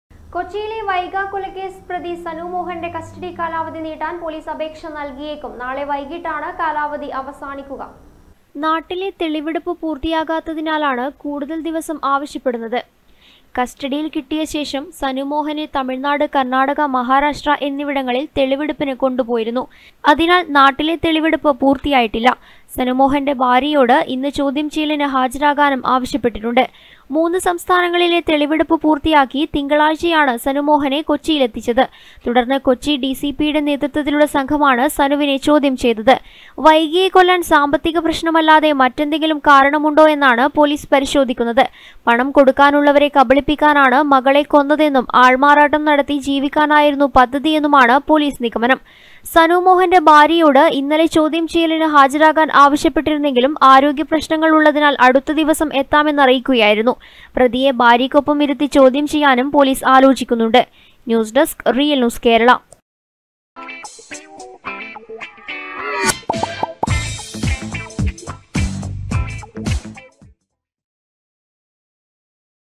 സനുമോഹന്റെ കസ്റ്റഡി കാലാവധി നീട്ടാൻ പൊലീസ് അപേക്ഷ നൽകിയേക്കും
കൊച്ചിയിലെ വൈഗ കൊലക്കേസ് പ്രതി സനുമോഹന്റെ കസ്റ്റഡി കാലാവധി നീട്ടാൻ പൊലീസ് അപേക്ഷ നൽകിയേക്കും. നാളെ വൈകിട്ടാണ് കാലാവധി അവസാനിക്കുക